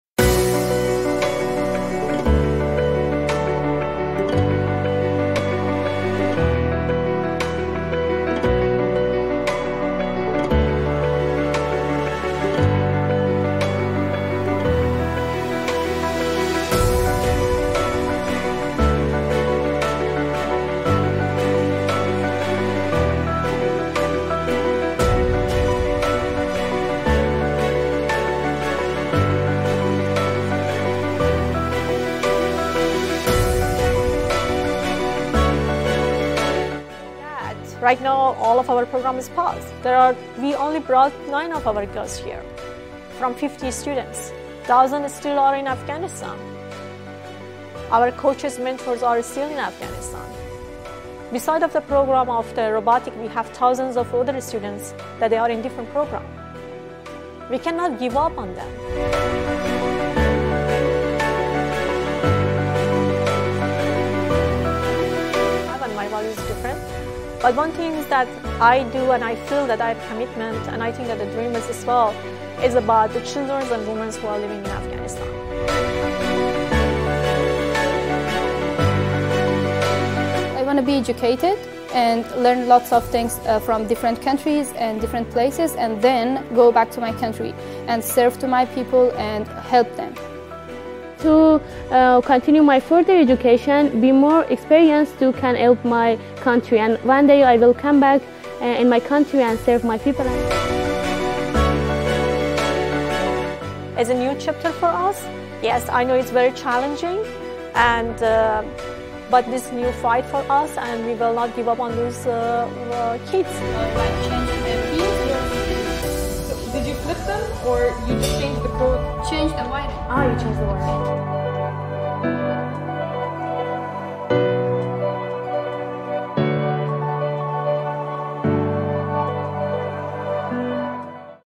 فتيات أفغانيات يصمّمن المستقبل في قطر! 🇦🇫
"الحالمات".. فريق روبوتات أفغاني مكون من النساء يشق طريقه في قطر! 🇦🇫